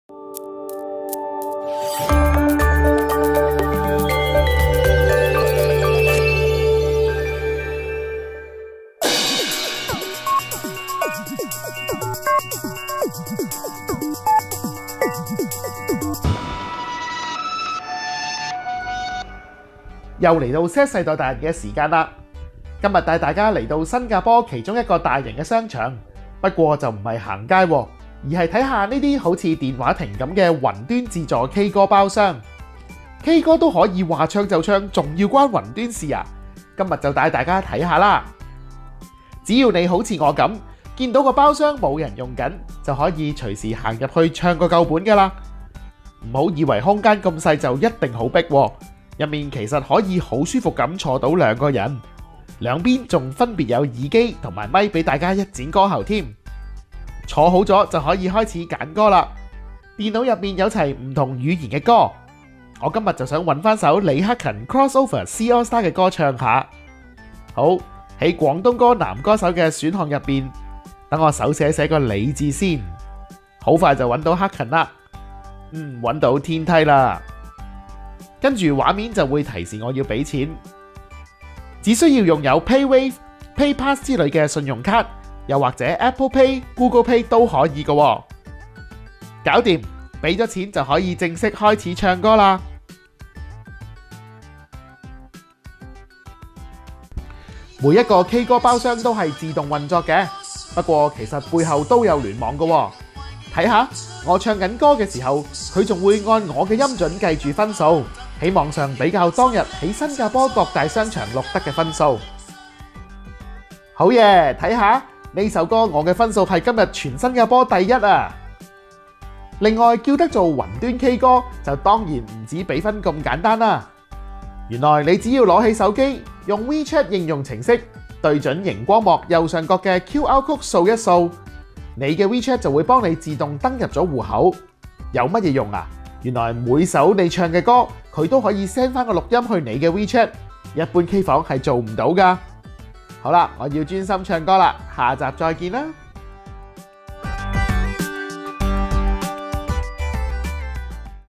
《Z世代達人》雲端自助K歌包廂
最新嘅科技資料、潮流玩意，都有《Z世代達人》麥卓華到處搜羅送到你面前！

今日帶大家來到新加坡其中一個大型商場，不過唔係行街，而係睇下呢D好似電話亭咁嘅「雲端自助K歌包廂」。K歌都可以話唱就唱，仲關雲端事？今日就帶大家去睇睇！

只要你好似我咁，見到無人用緊個包廂，就可以隨時行入去唱過夠本。唔好以為空間咁細就一家好迫呀，入面其實可以好舒服咁坐兩個人，兩邊仲分別有兩個耳機同埋兩支咪俾大家一展歌喉！坐好左就可以開始揀歌，電腦入面有齊唔同語言嘅歌， 跟住畫面就會提示你俾錢，你只需要用有 Paywave / Paypass 之類嘅信用卡，又或者ApplePay / GooglePay 就可以。俾咗錢，就正式可以唱歌喇！

每一個 K歌包廂都係自動運作，不過其實背後都有聯網，唱緊歌嘅時候佢會按音準計分，在網上比較當日在新加坡各大商場所錄得嘅分數。另外，叫得做雲瑞K歌，就當然唔只俾分咁簡單！原來只要用你手機嘅WeChat 應用程式對準螢光幕右上角既QR Code掃一掃，你就會自動用Wechat戶口登入左佢地嘅系統，有乜功能？ 原來每首你唱左嘅歌，佢都可以send番錄音去你嘅WeChat，一般K房都做唔到架！

《Z世代達人》
主持：麥卓華